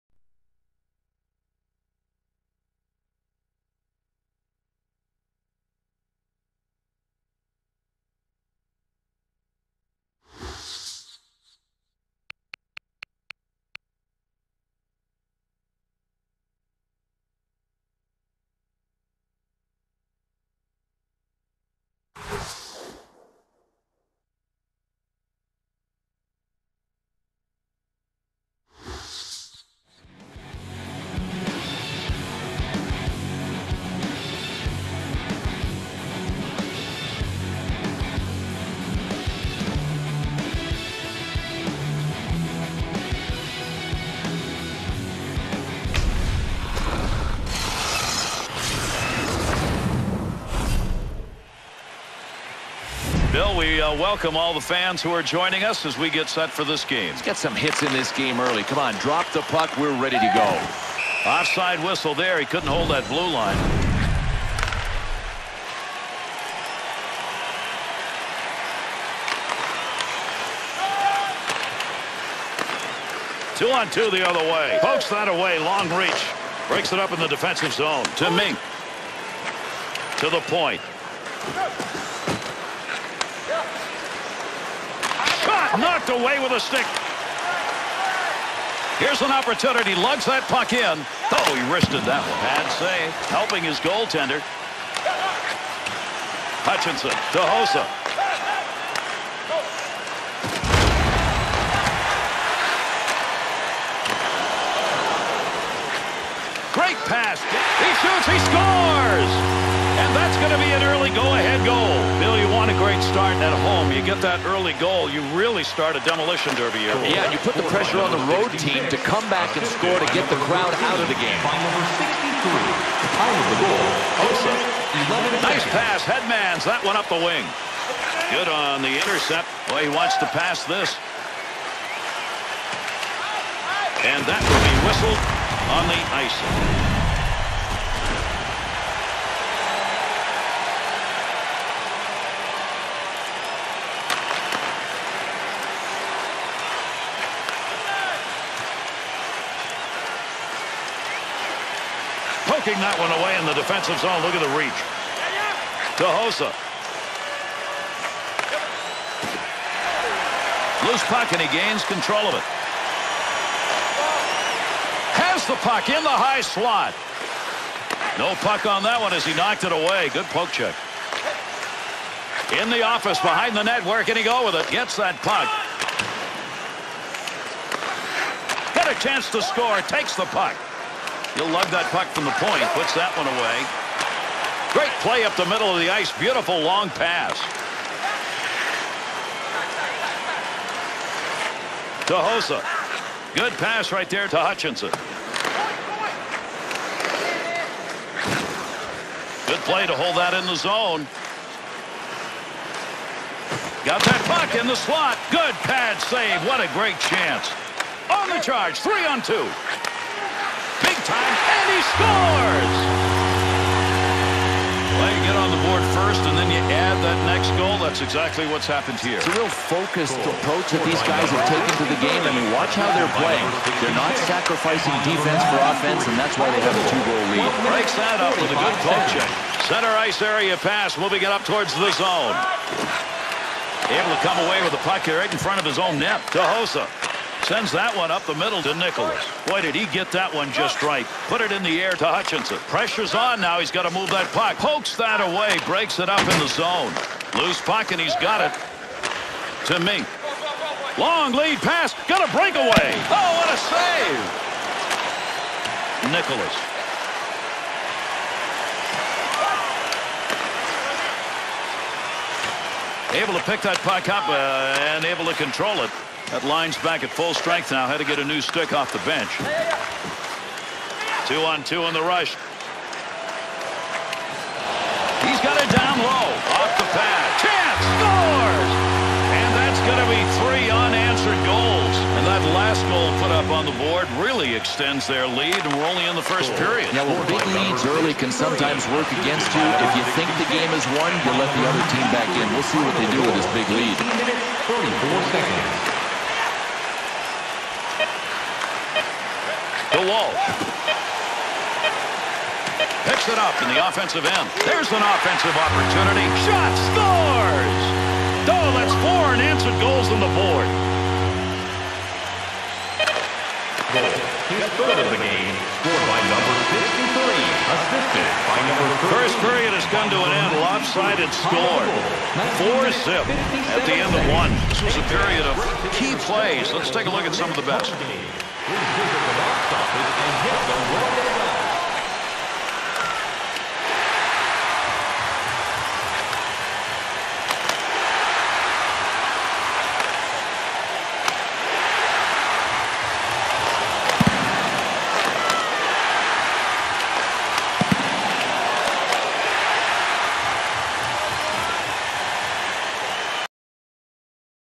ANA vs PHI
VHL Regular Season
Anaheim Ducks vs Philadelphia Flyers
11/14/11 - 9pm